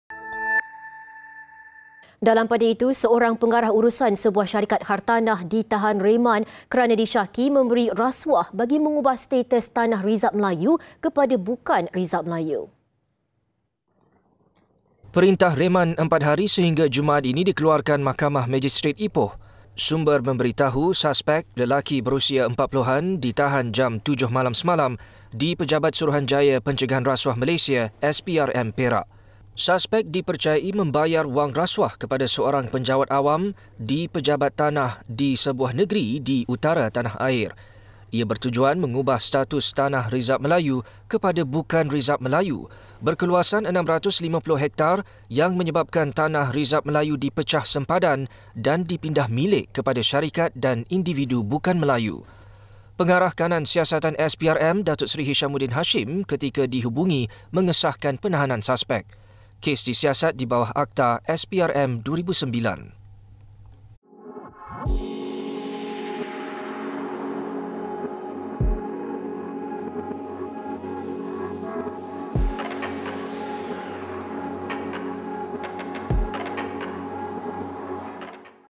KES RASUAH | Pengarah Urusan Disyaki Beri Suapan Ubah Status Tanah Rizab Melayu
Seorang pengarah urusan sebuah syarikat hartanah ditahan reman kerana disyaki memberi rasuah bagi mengubah status tanah rizab Melayu kepada bukan rizab Melayu.